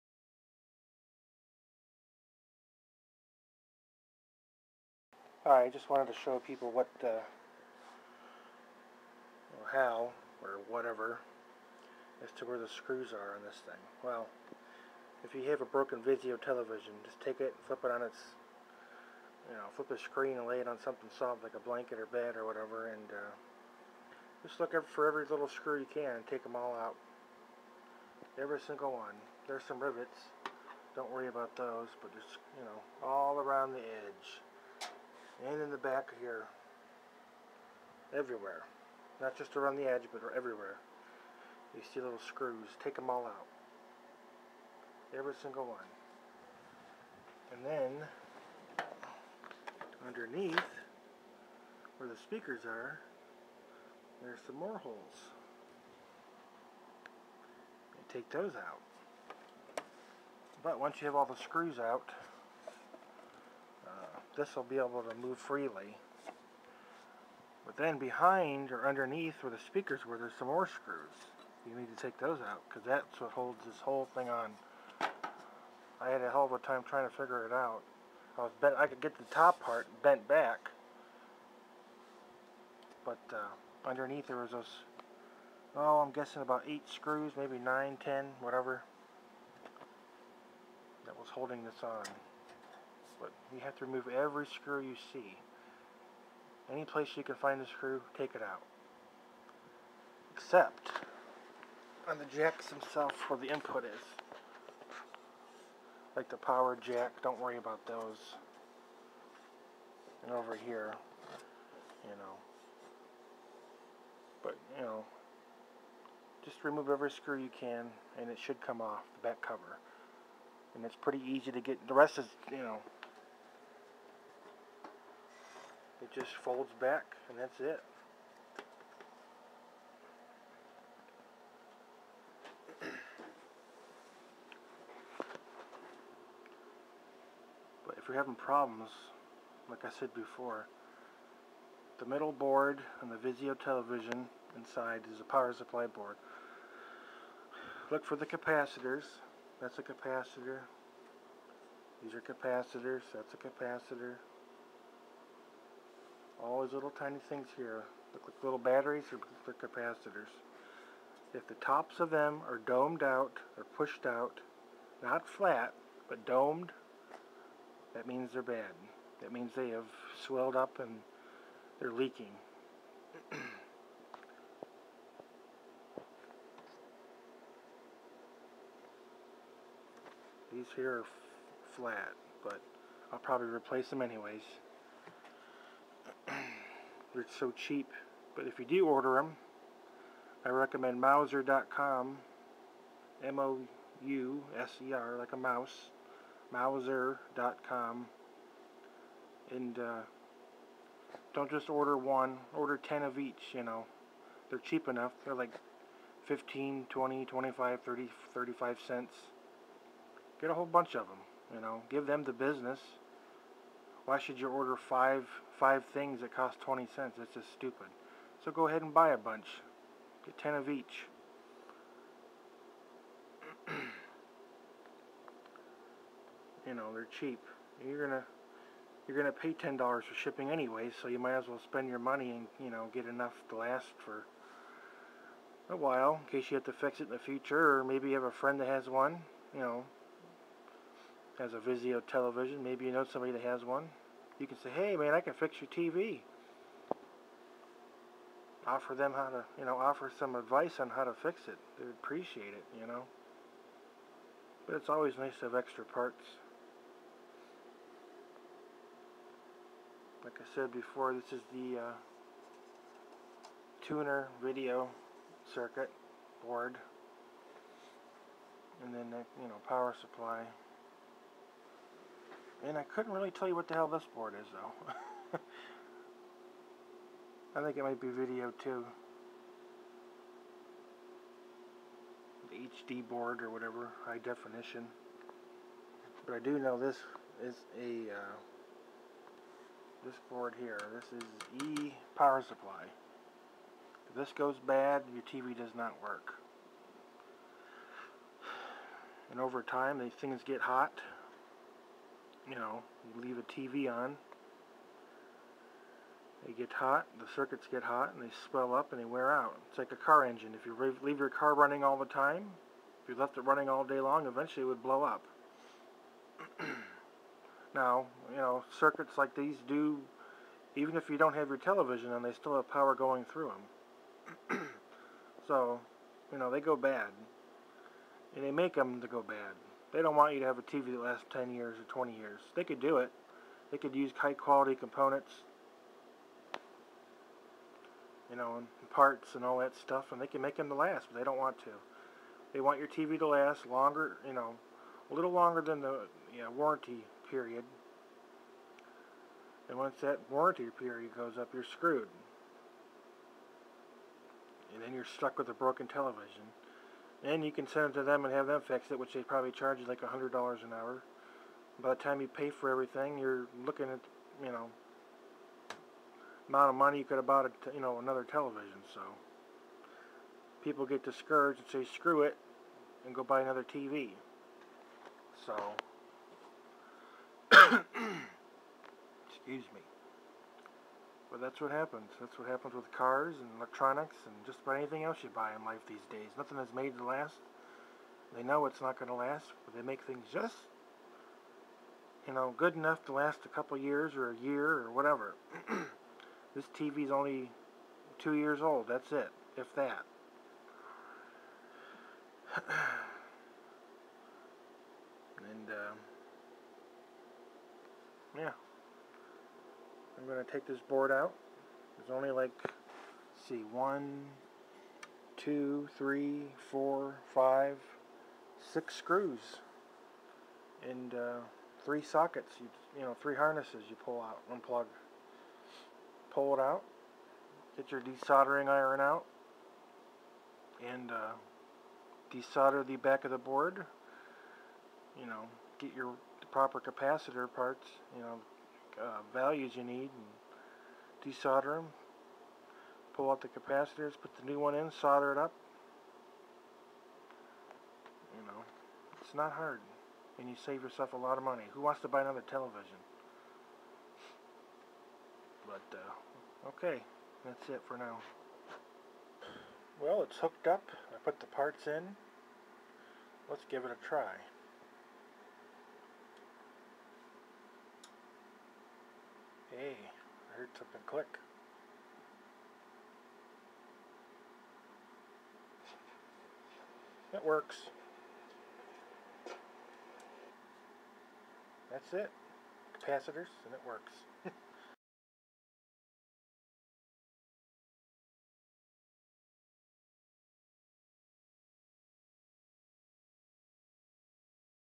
Vizio VP322 HDTV repair tips and information (part 2 of 2)
If your VIZIO VP322 HDTV or similar models starts having trouble turning on or is completely dead, watch this video. I offer some repair tip information. ALSO, if you do not feel comfortable repairing electronics yourself, I offer repair service in the United States for a reasonable price.

Parts list: get 10 of each to be on the safe side.

1000uf 10v Electrolytic capacitor (radial lead)

3300uf 10v Electrolytic capacitor (radial lead)

1000uf 25v Electrolytic capacitor (radial lead)

470uf 10v  Electrolytic capacitor (radial lead)

1000uf 10volt Mouser part # 667-EEU-FR1A102L
3300uf 10volt Mouser part # 667-EEU-FM1A332
1000uf 25volt Mouser part # 667-EEU-FC1E102S
470uf   10volt Mouser part # 667-ECA-1AM471